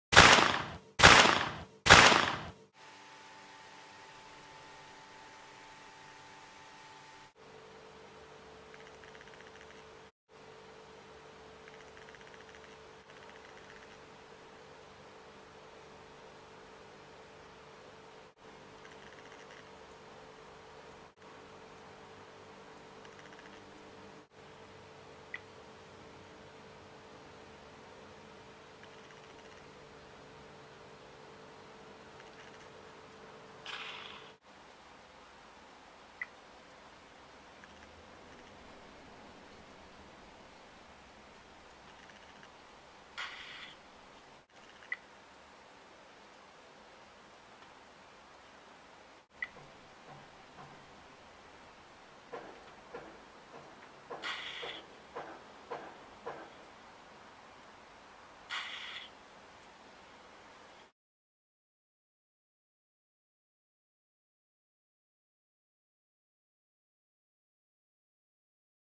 Gun Movie FX on iPhone, iPad or iPod Touch 4&5G
Gun Movie FX app, quick and simple way to create a gunshot FX movie on your iPhone, iPad or iPod 4th & 5th Generation, as shown in this tutorial.